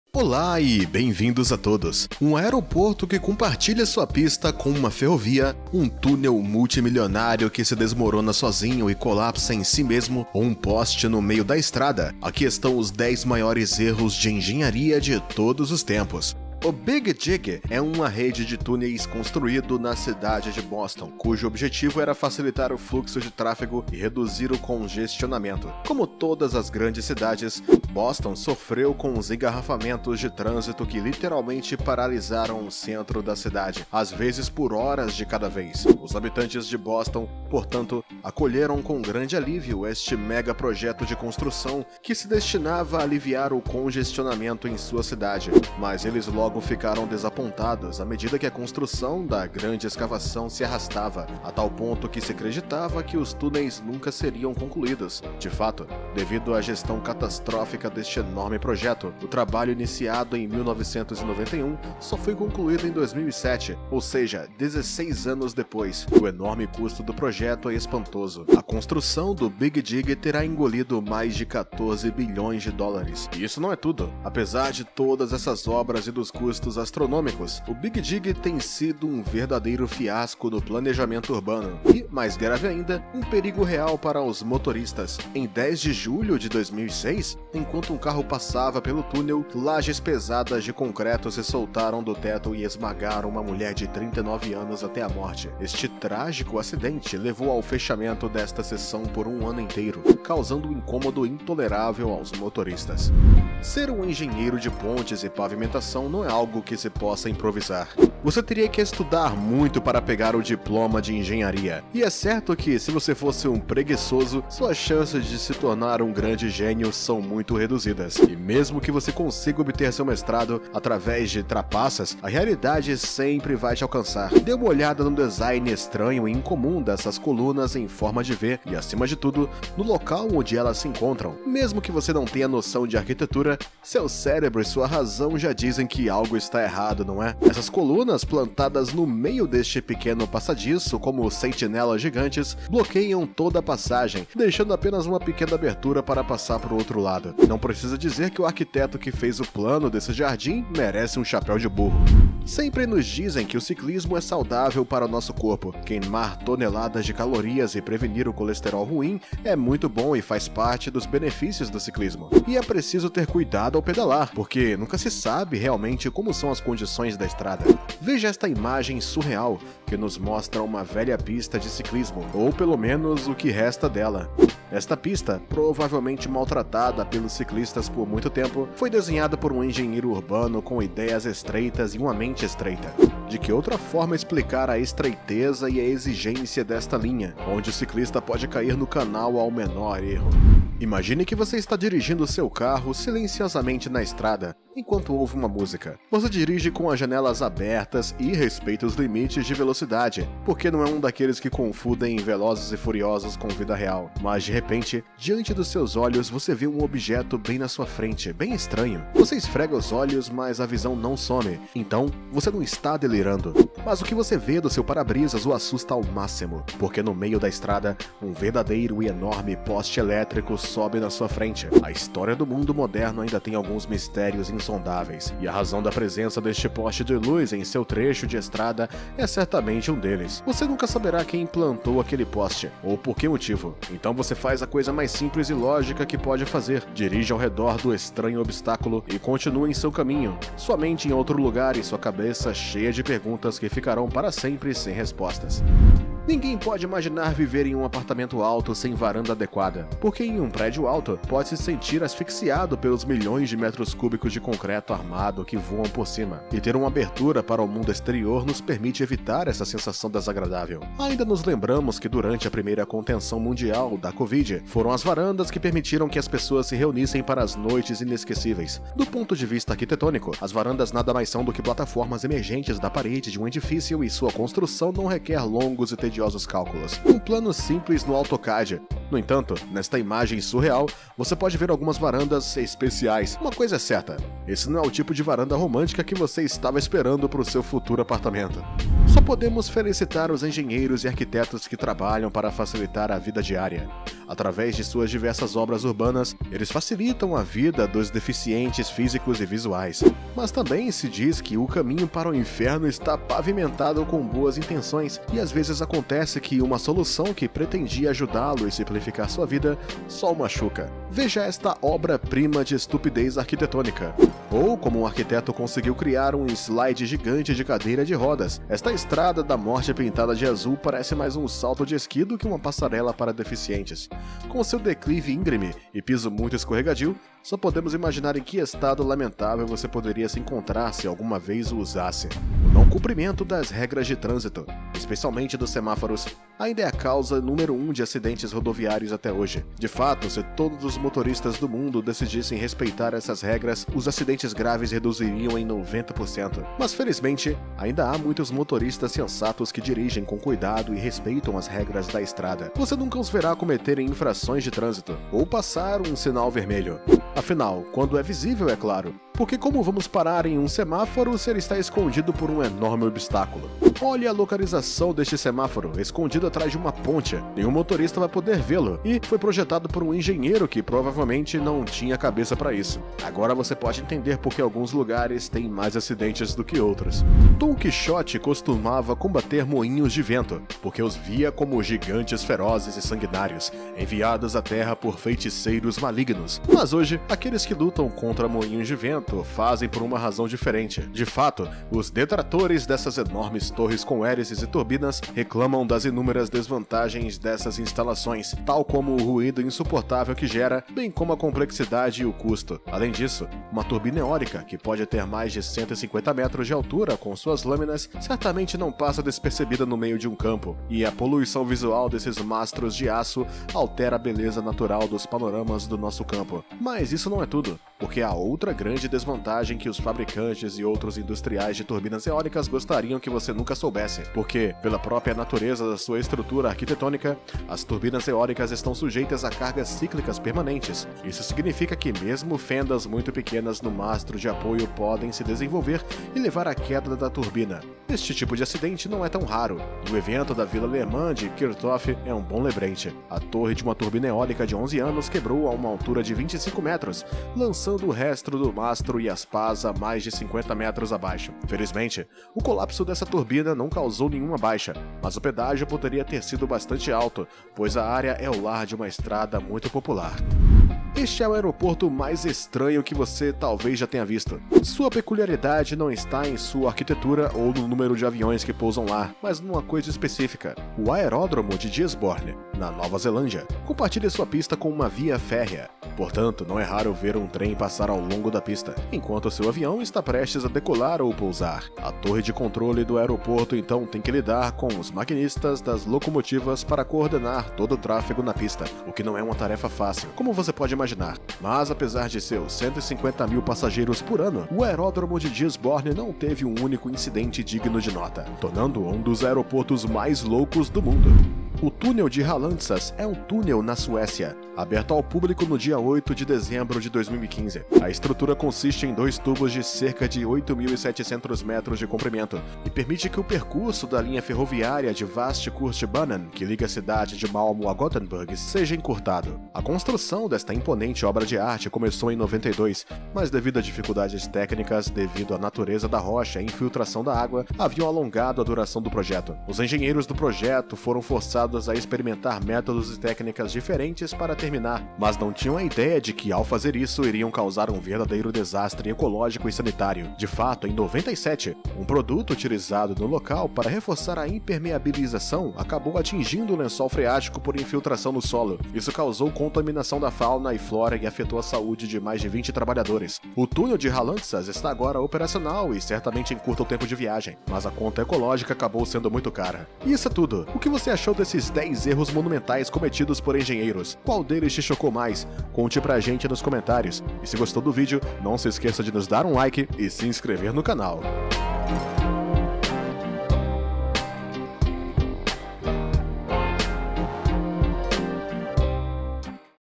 Estes Erros Da Engenharia Vão Surpreendê lo
Um aeroporto que compartilha sua pista com uma ferrovia, um túnel multimilionário que se desmorona e colapsa sobre si mesmo, ou um poste no meio da estrada - estes são os 10 maiores erros de engenharia de todos os tempos!